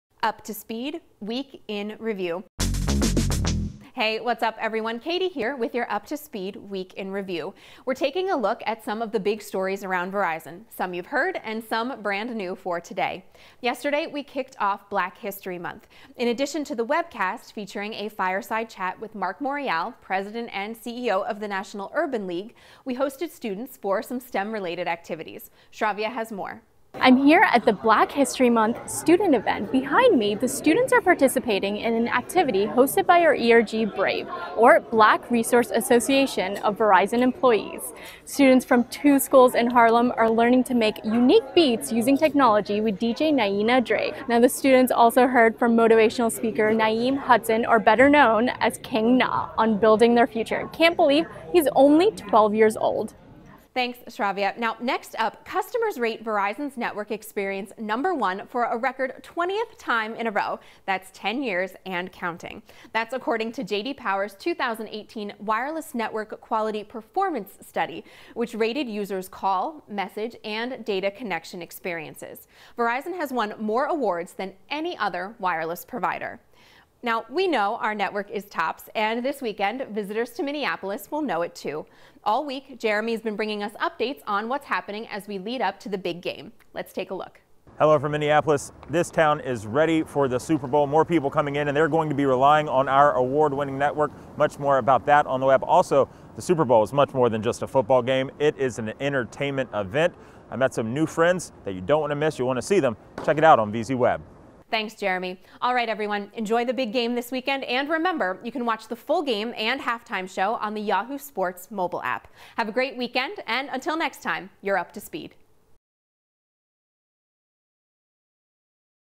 Up to Speed, February 2.
What’s happening around Verizon? It’s our week in review recapping the kickoff to Black History Month, our preparations for Super Bowl LII, and our latest network win.